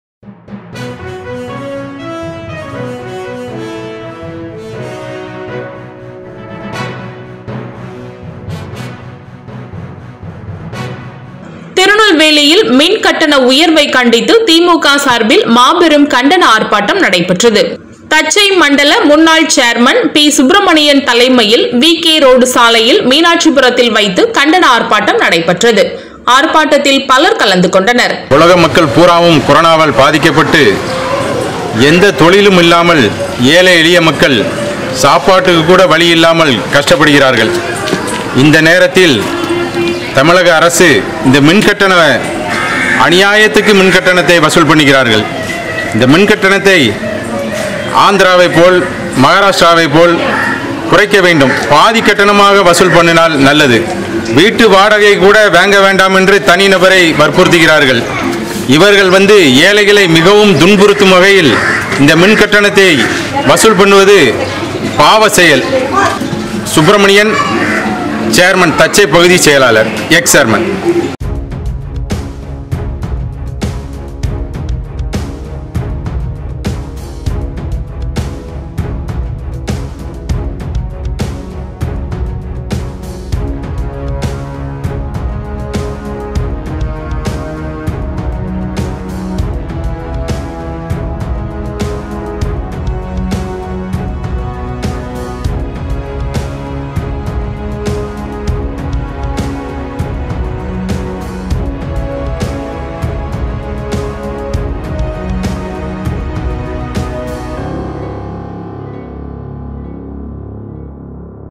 மின் கட்டண உயர்வை கண்டித்து நெல்லையில் கண்டன ஆர்ப்பாட்டம் நடைபெற்றது
மின் கட்டண உயர்வை கண்டித்து நெல்லையில் தச்சை மண்டல முன்னாள் சேர்மன் சுப்ரமணியன்  தலைமையில் கண்டன ஆர்ப்பாட்டம் நடைபெற்றது